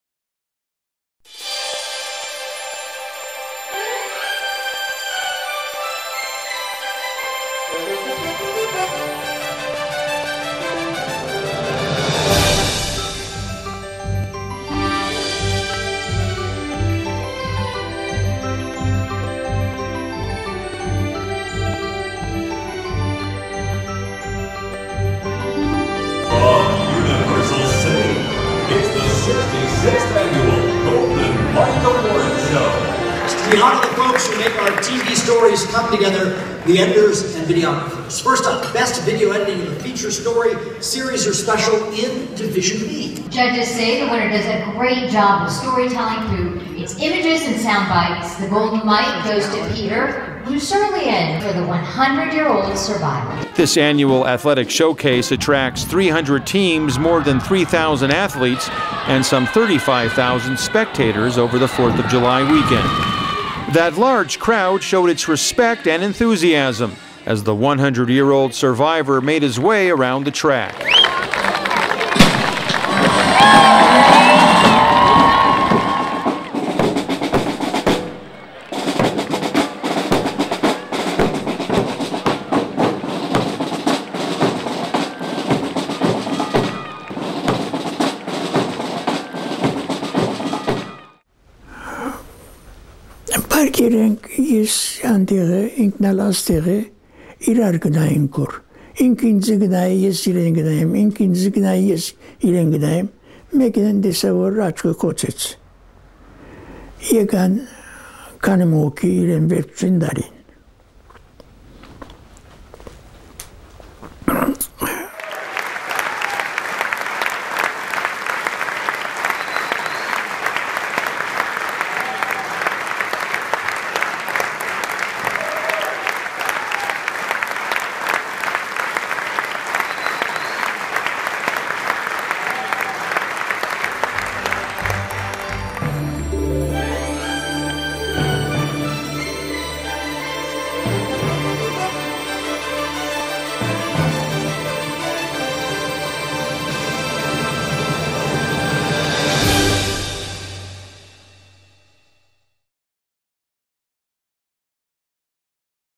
Horizon Armenian TV wins a 2015 RTNA Golden Mike - (January 2016)
Globalist Films & Horizon Armenian TV team-up to win a Golden Mike for "THE 100-YEAR-OLD SURVIVOR."  The award [in Division B], for stations with fewer than 50 employees, was in category TV-29 : BEST VIDEO EDITING OF A FEATURE STORY, SERIES OR SPECIAL.

It was Horizon's first Golden Mike, but the 21st for Los Angeles Area Emmy Award-winning journalist Peter Musurlian, who has won the other Golden Mikes, during a nearly 20-year career at The Burbank Channel.

For a complete list of awards, go to the Linkedin page of Peter Musurlian.

The RTNA is the Radio & Television News Association of Southern California